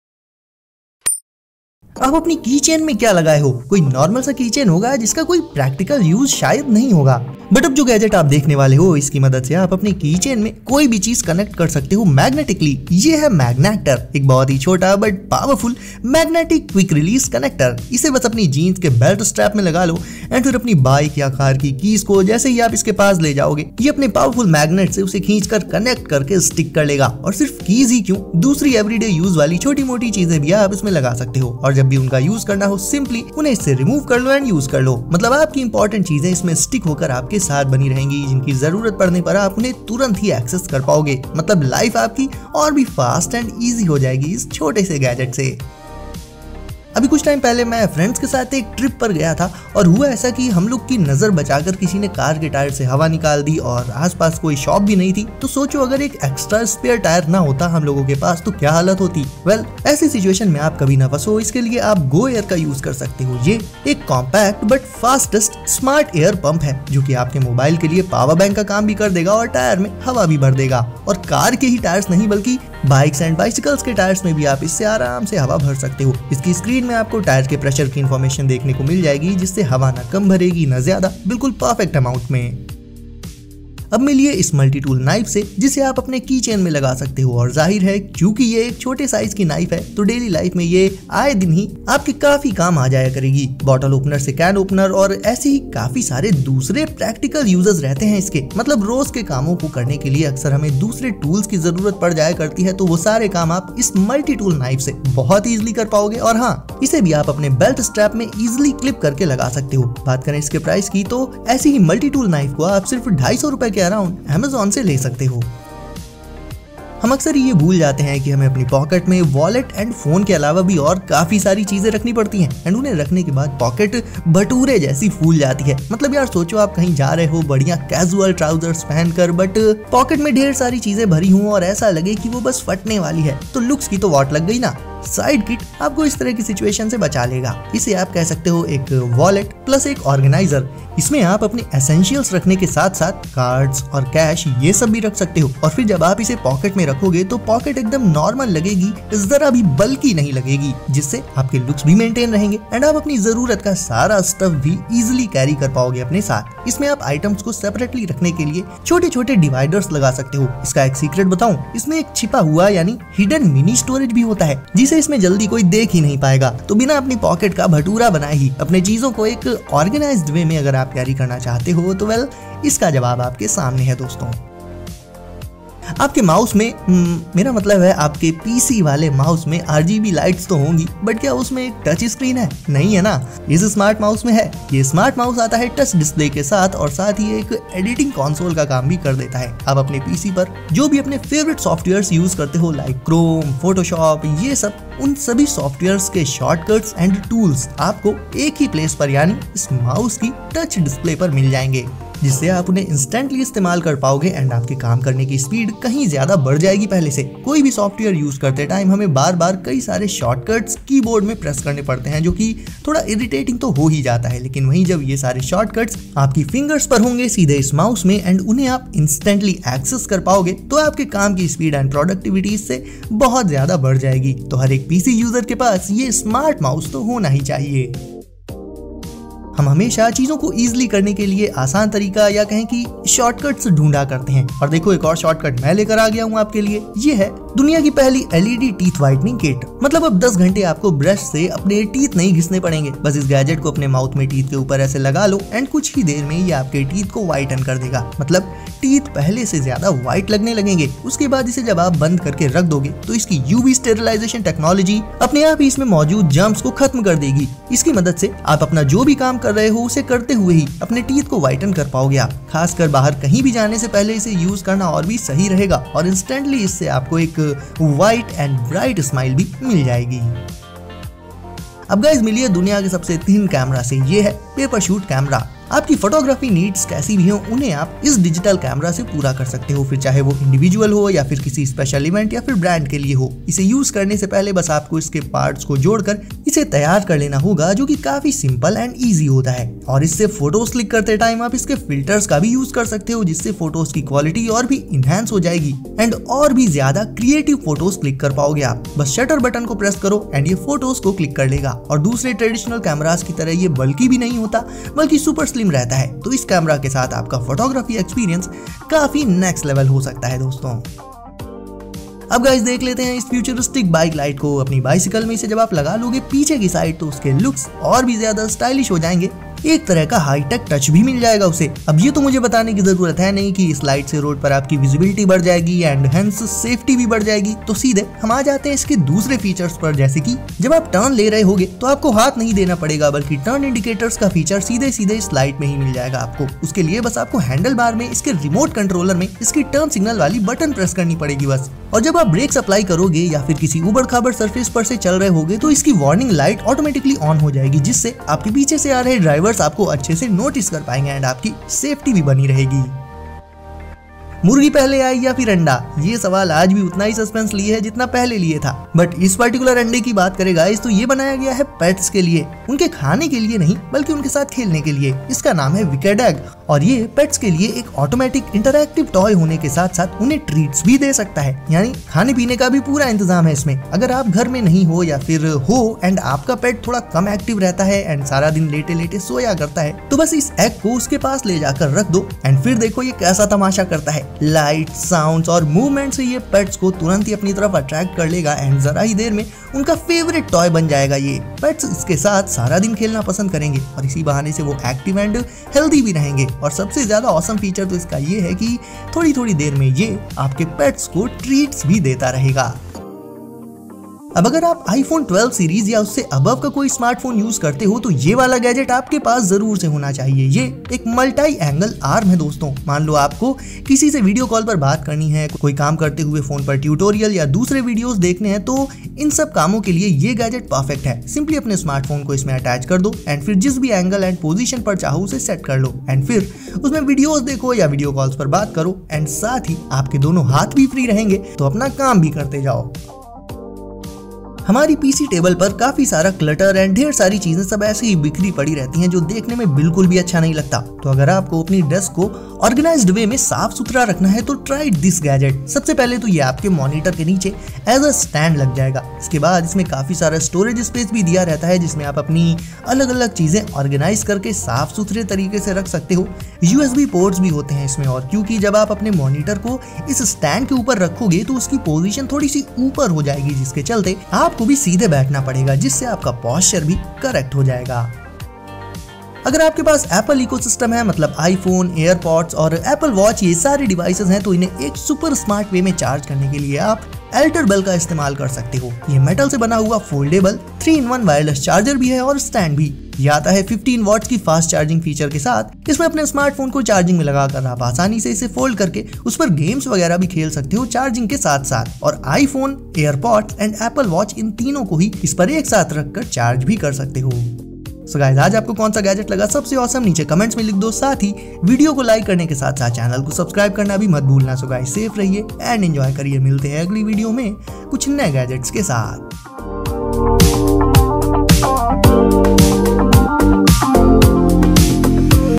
12 COOL GADGETS FOR BOYS ON AMAZON | Gadgets from Rs.100, Rs200 and Rs1000
Topics Covered- 12 COOL GADGETS FOR BOYS ON AMAZON

Gadgets Links-

1. Quick Release Keychain

2. Tire Inflator

3. EDC Mini Knife

4. Organizer Mini Bag

5. Budget Mouse

6. Teeth Whitening device

8. Bicycle Lights

9. Pet toys

11. MOOJAY Monitor Stand for Desk